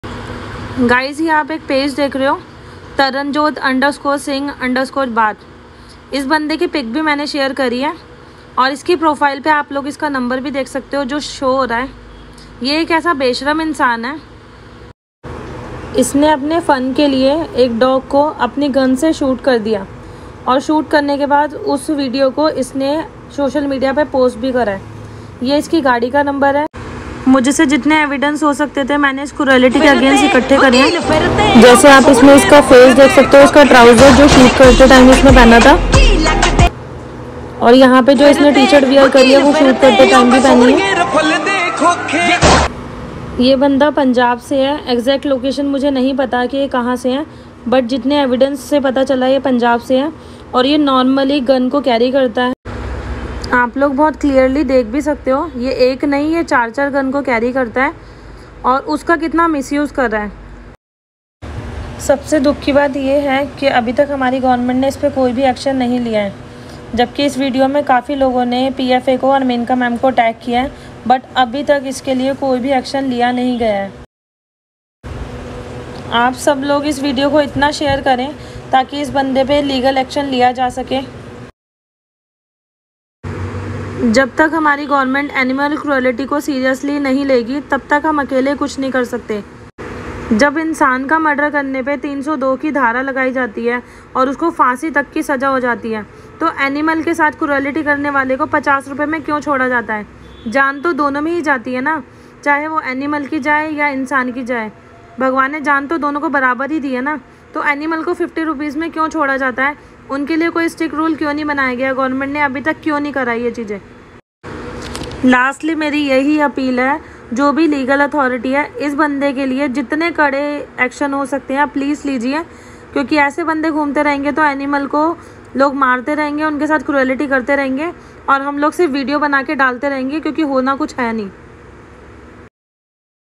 thinkpawsitiveanimal doglover
Guys see this video this person shooted a dog just for fun.now i don't know what is current situation of dog he is alive or not I got this video through Instagram where someone tagged him to his Instagram id taranjot_singh_baath his contact number is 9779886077.so i am requesting all of you to punished him because I think he is a black spot on the name of human.i don't why the people reacting very cruel for the voiceless who is unable to express their feeling.